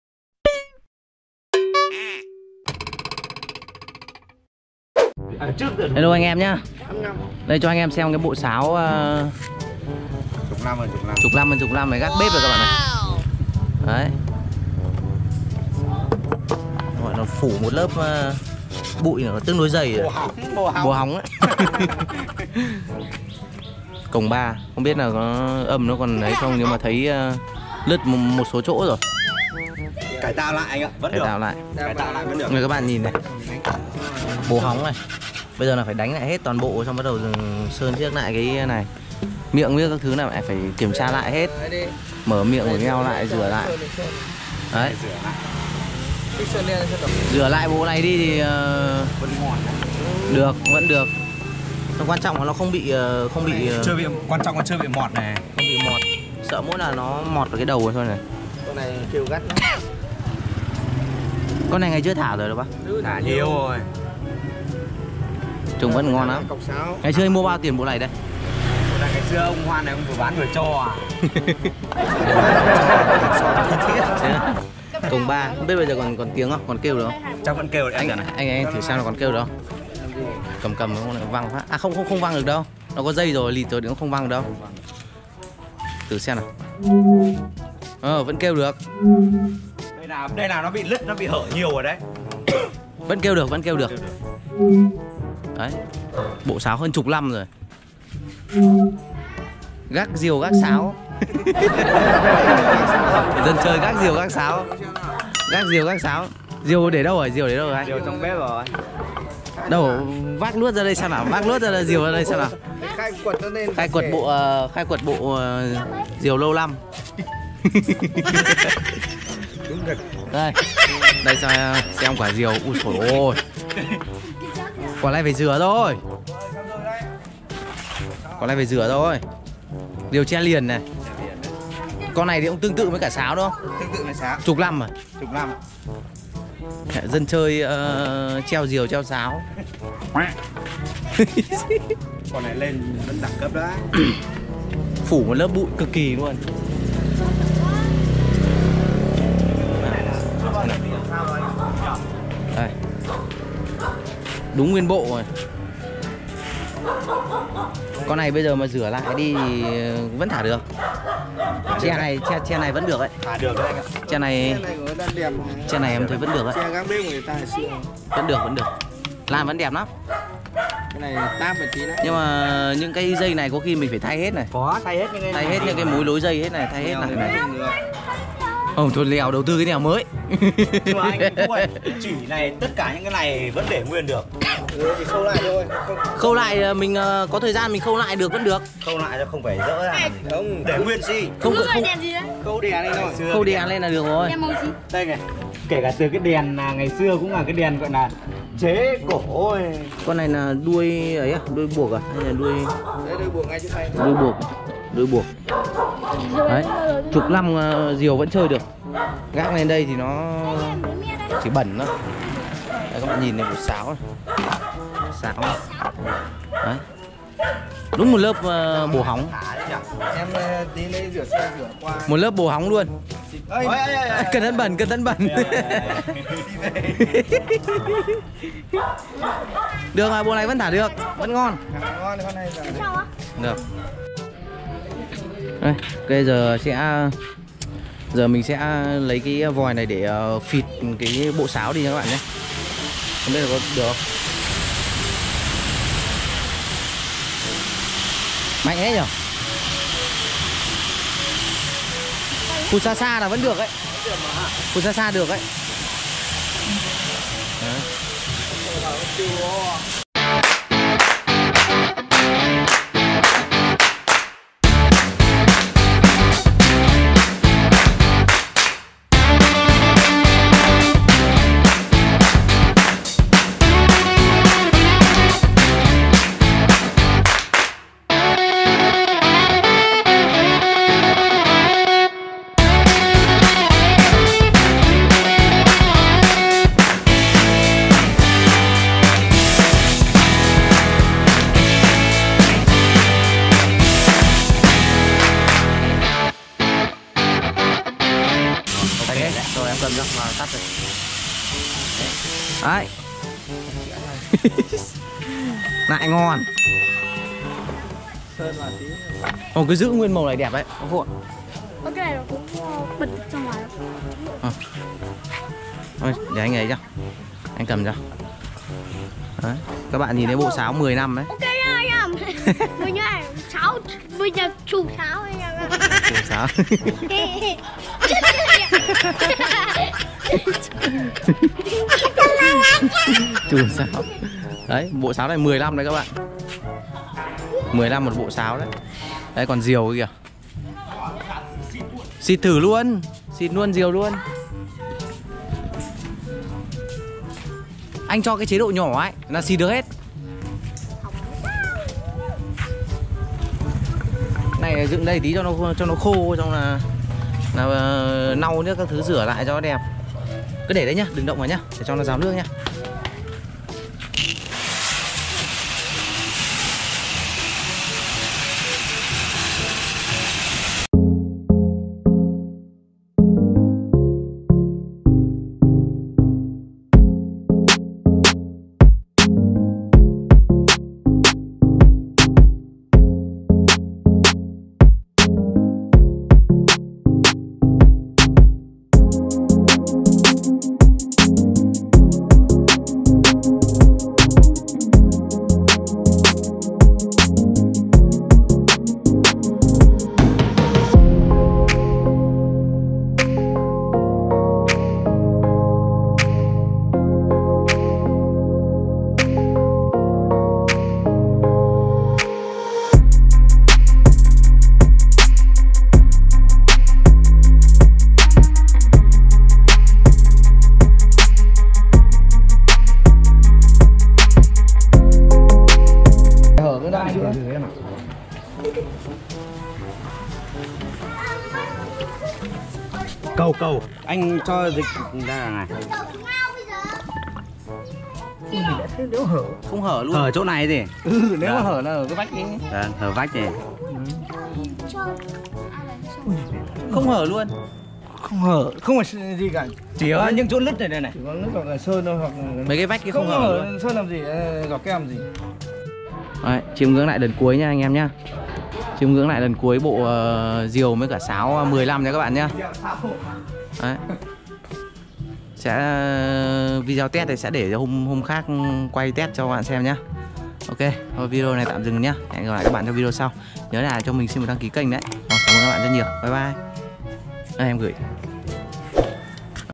Bộ Sáo Diều 10 Năm Tuổi Liệu Có Thể Bay II Huy Bình Vlogs
Các bạn đã bao giờ thấy món này chưa ( Diều Gác Bếp ) Liệu Sau 10 Năm em ý có thể cất cánh bay dc
AE hãy chờ đón xem nhé.